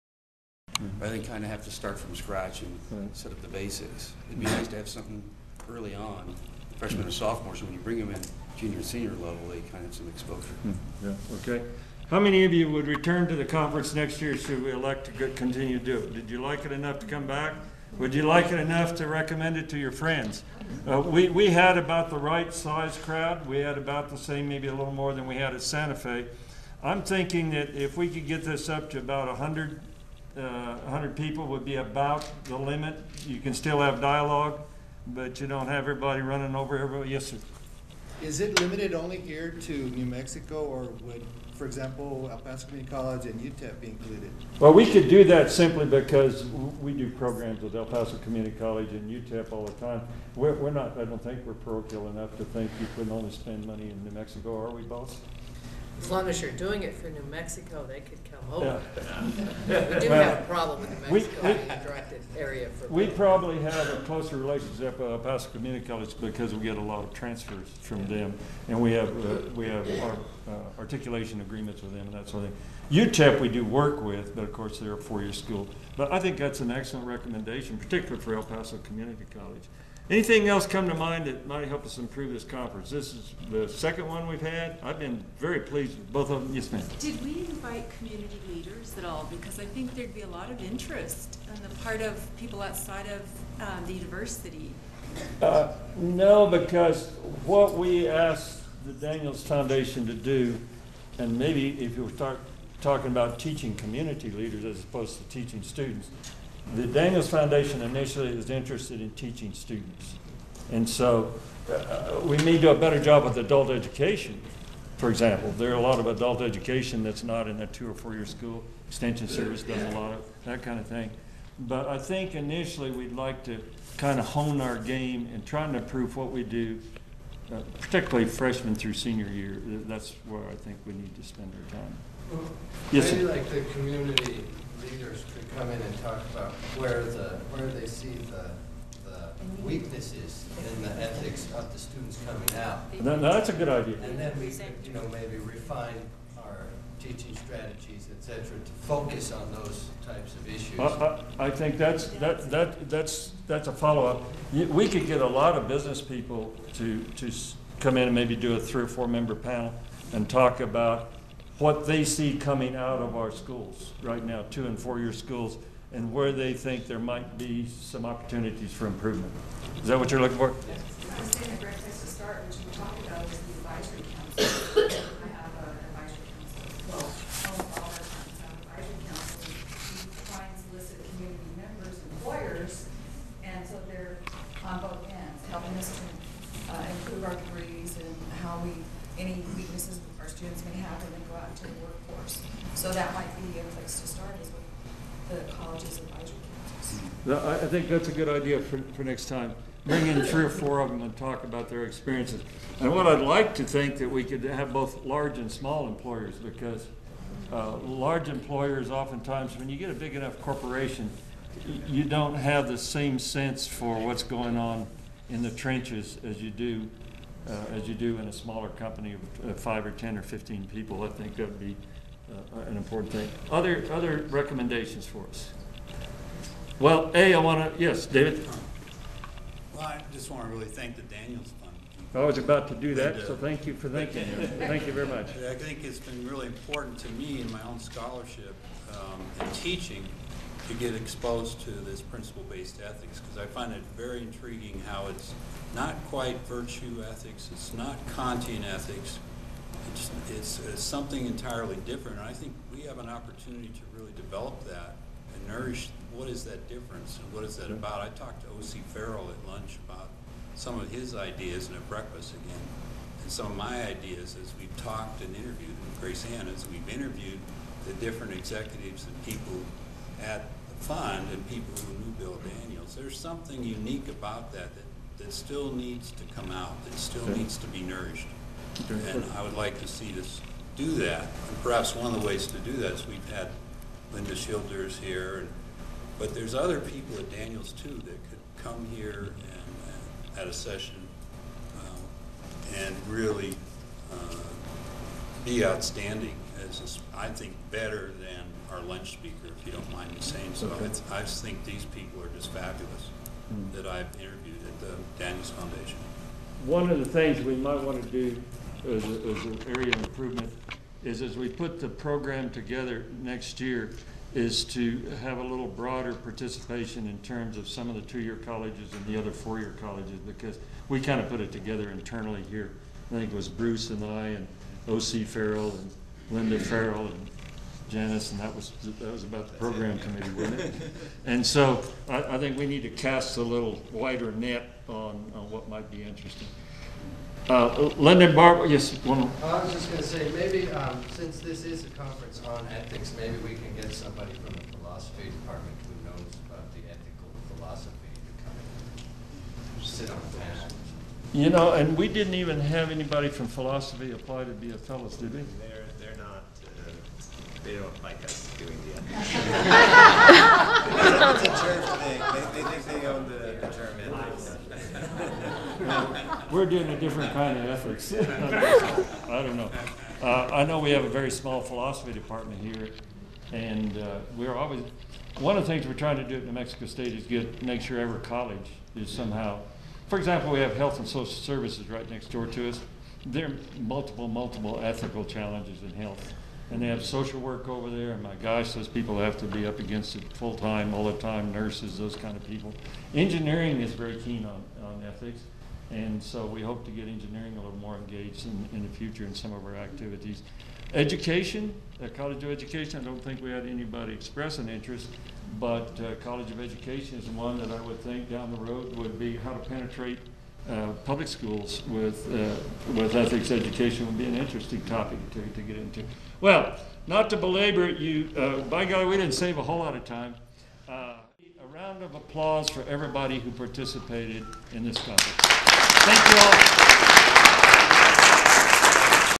26 Ethics Workshop 1-6-2011 Conference Evaluation-Pt 2
January 6-7, 2011: Garrey E. Carruthers, Dean of the College of Business at New Mexico State University, conducts an evaluation of the first NMSU-UNM Teaching Business Ethics workshop. Part 2.

Subject Topic:  Evaluation
Discipline:  Business ethics
Industry:  Education

Type of Resource: video
University Name: New Mexico State University
Date Created: January 7, 2011
Author: Bruce A. Huhmann